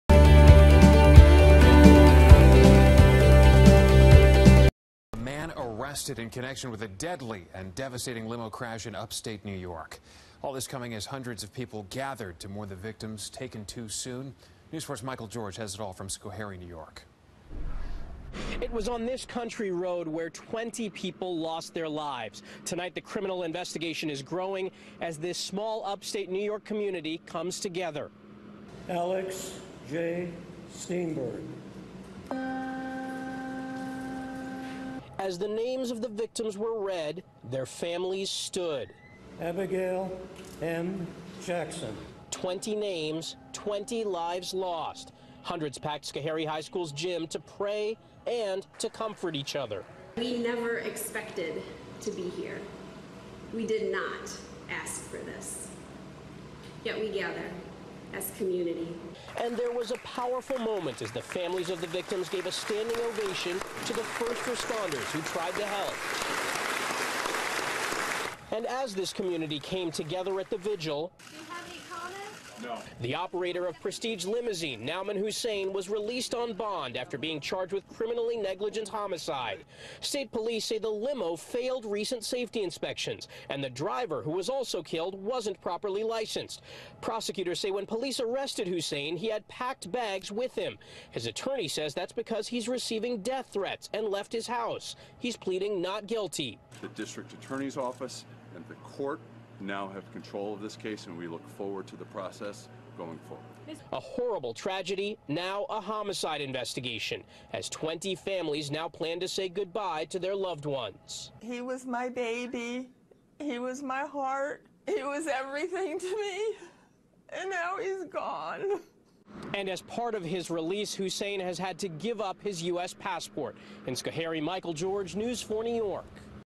Limo Company Operator Charged in Wreck That Killed 20 People
The operator of the company involved in the deadly limousine crash that killed 20 people in New York is free on bail after pleading not guilty Wednesday to a single count of criminally negligent homicide. Nauman Hussain, 28, could face more charges